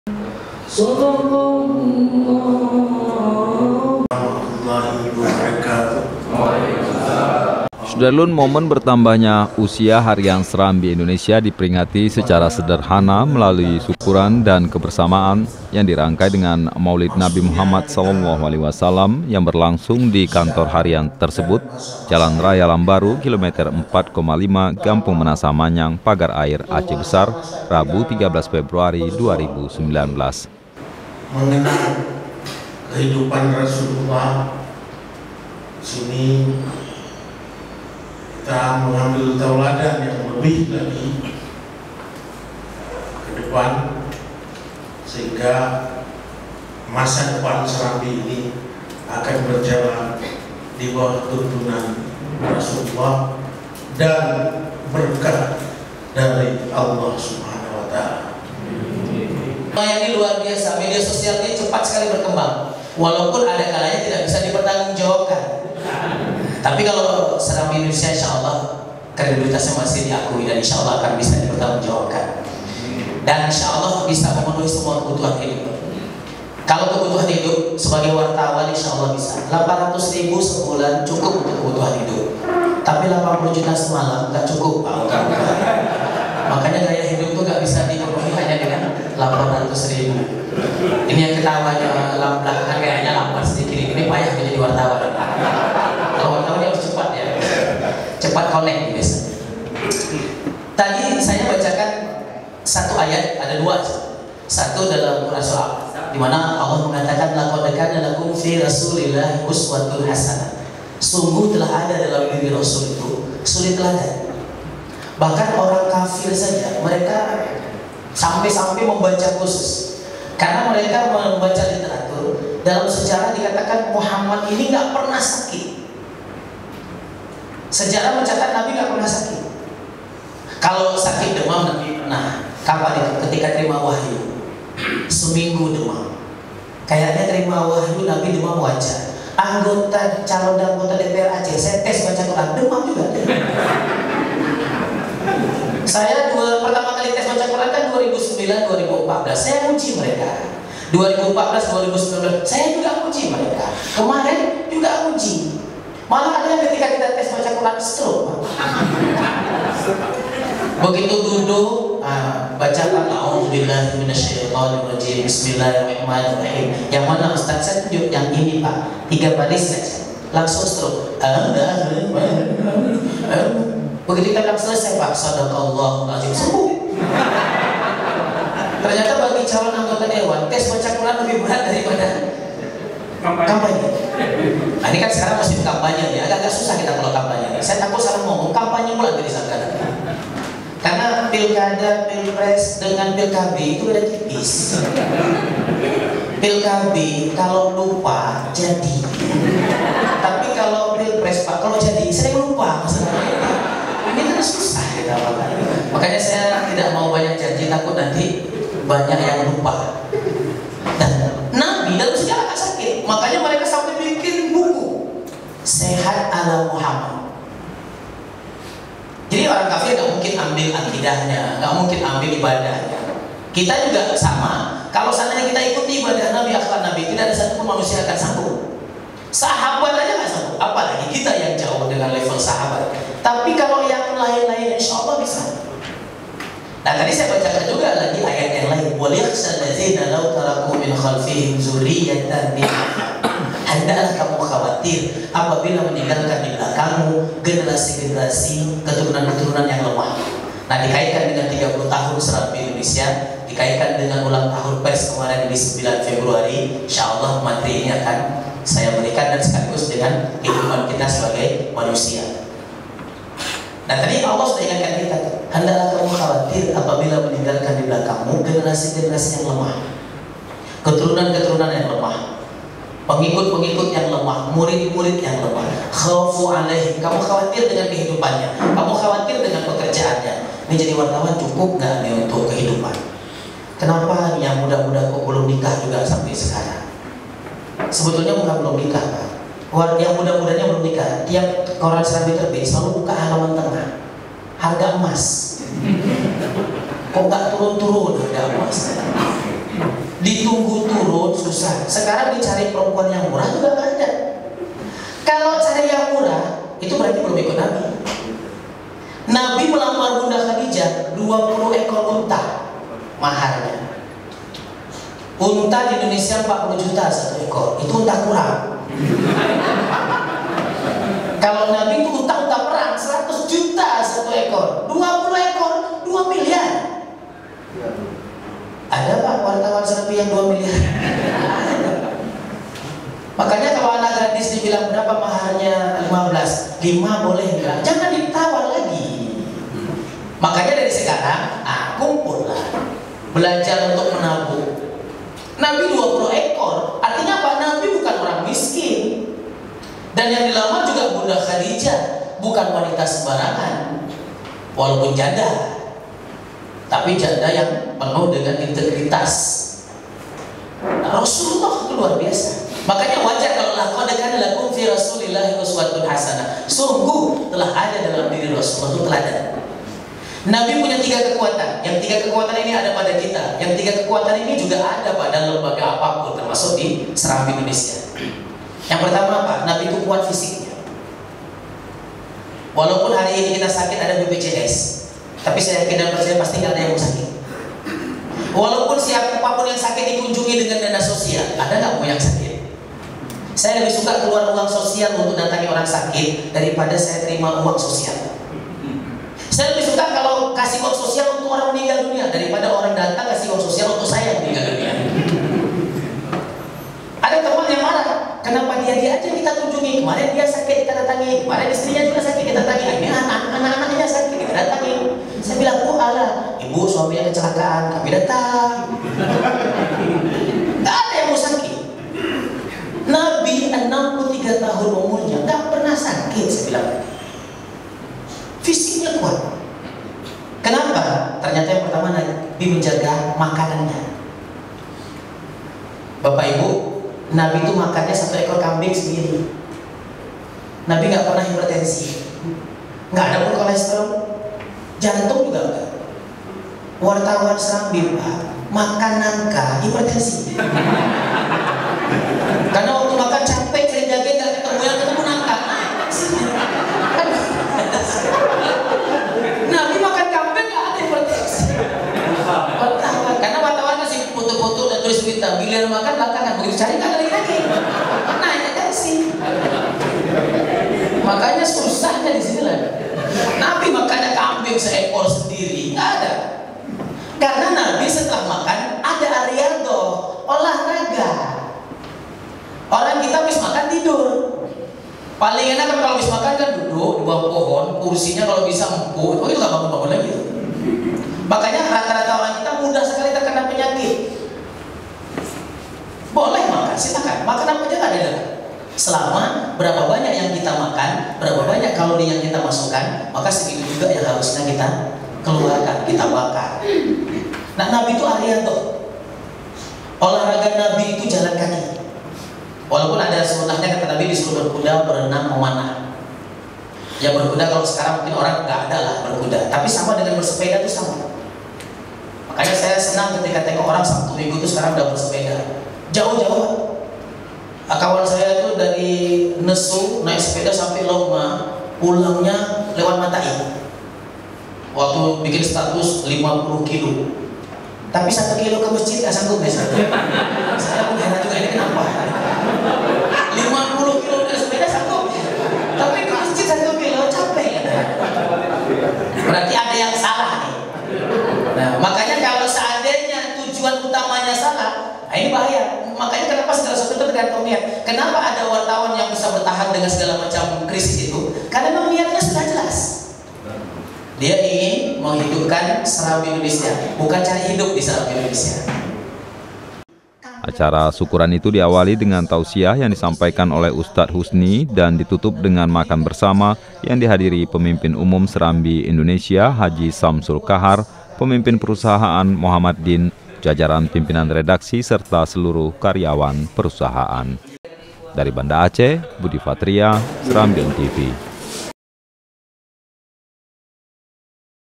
Peringatan Maulid dan Syukuran 30 Tahun Serambi Indonesia
SERAMBITV.COM, BANDA ACEH - Harian Serambi Indonesia yang terbit sejak 9 Februari 1989 kini genap berusia 30 tahun.

Momen bertambahnya usia Harian Serambi Indonesia diperingati secara sederhana melalui syukuran dan kebersamaan yang dirangkai dengan Maulid Nabi Muhammad SAW yang berlangsung di kantor harian tersebut, Jalan Raya Lambaro Km 4,5, Gampong Meunasah Manyang Pagar Air, Aceh Besar, Rabu (13/2/2019).

Acara syukuran itu, diawali dengan tausiah yang disampaikan oleh Ustaz Husni dan ditutup dengan makan bersama yang dihadiri Pemimpin Umum Serambi Indonesia H Sjamsul Kahar, Pemimpin Perusahaan Mohd Din, jajaran pimpinan redaksi, serta seluruh karyawan perusahaan.